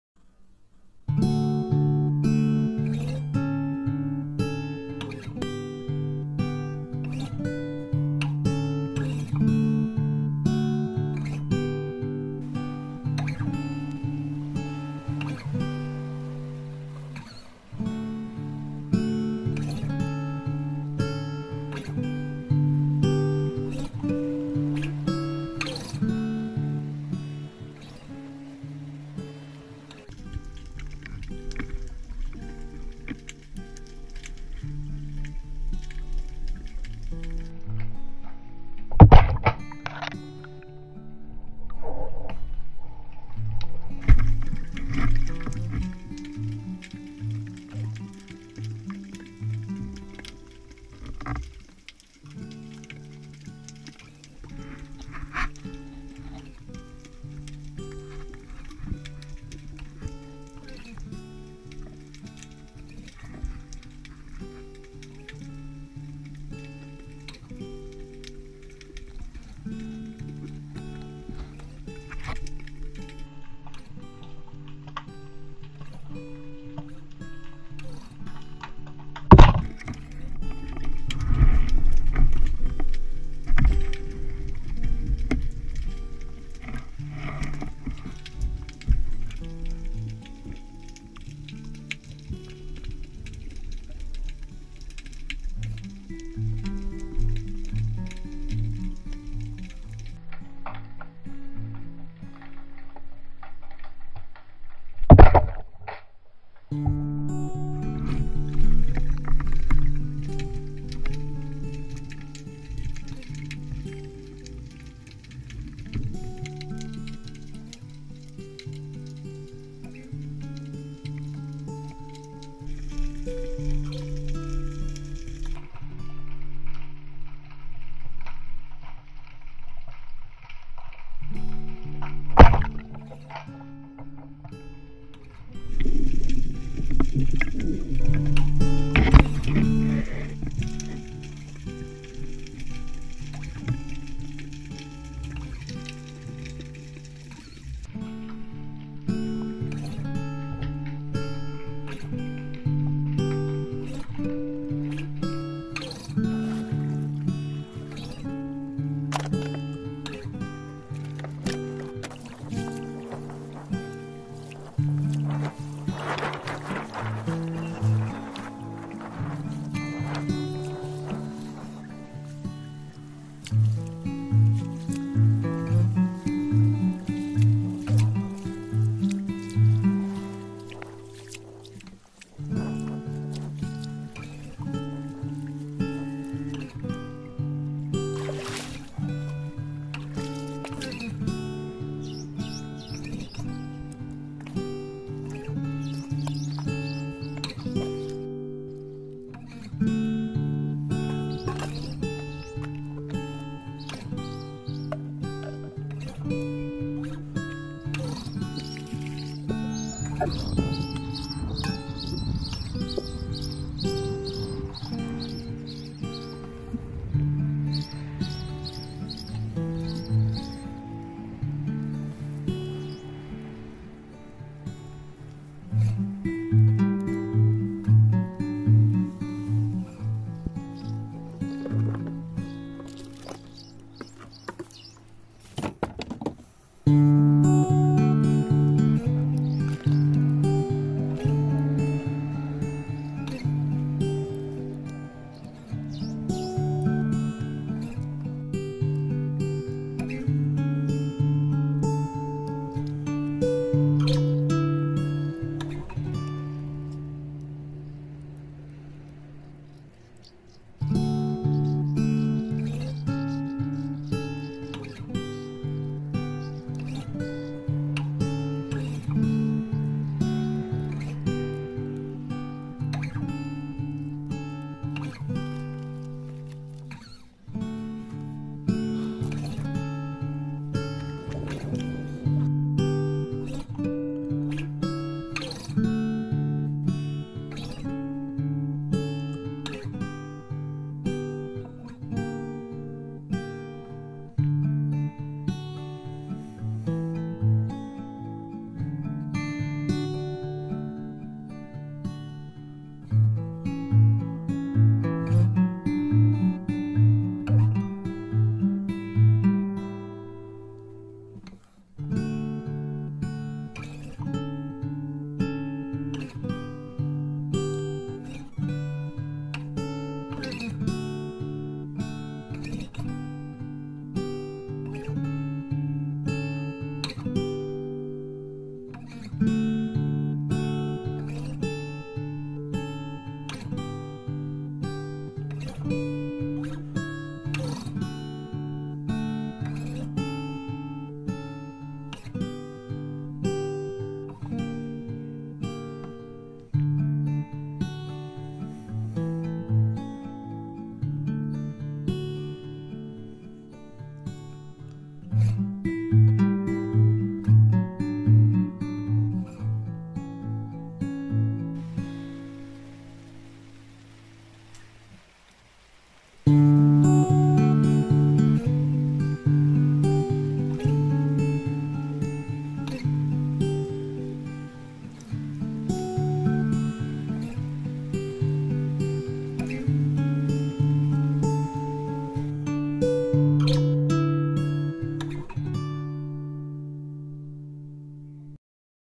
Zıpkınla balık avı sade ve güzel Ahmet maden spearfishing Catch and Cook
Gerçekten zor bir gündü. Bende biraz halsizdim. İki levreği kaçırdım günün sonunda iki ispari ve iki Karagöz almayı başardım. Planda olmayan bir şekilde suyun kenarında ateş yakıp pişirmeye karar verdik. Kader de bizden yana olunca her şey çok güzel tamamlandı.